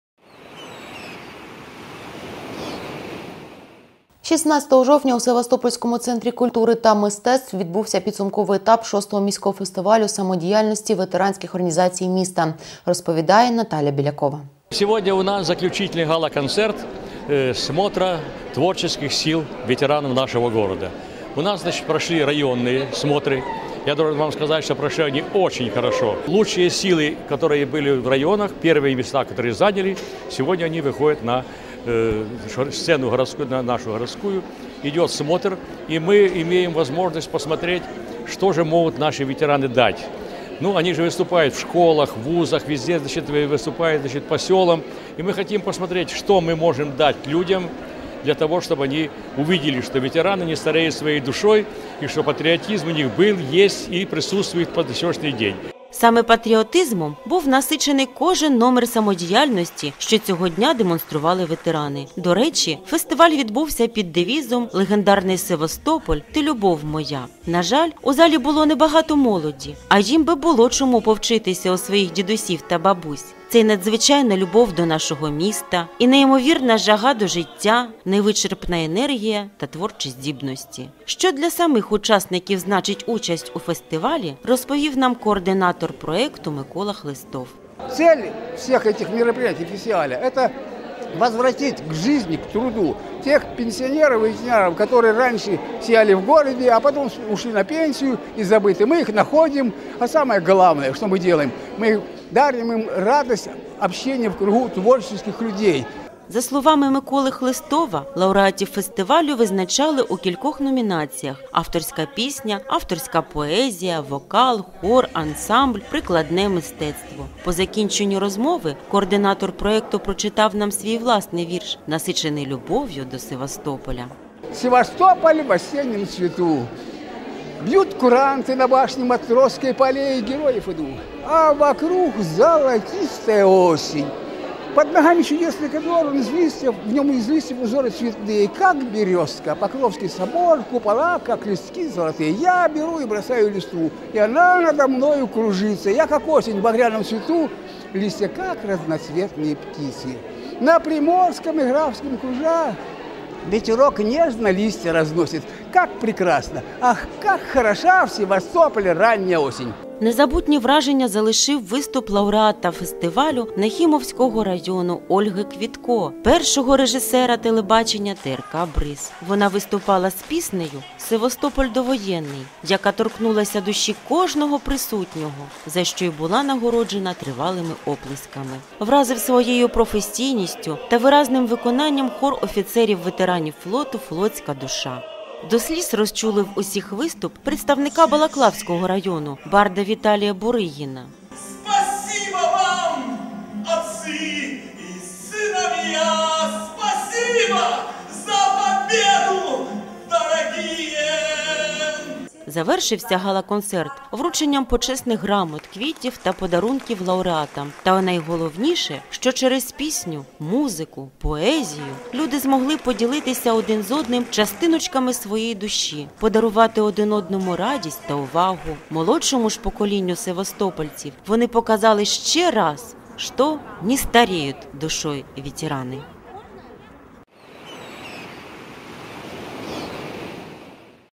Підсумковий етап 6-го Міського фестивалю самодіяльності ветеранських організацій
16 жовтня в Севастопольському Центрі культури та мистецтв відбувся підсумковий етап 6-го Міського фестивалю самодіяльності ветеранських організацій міста.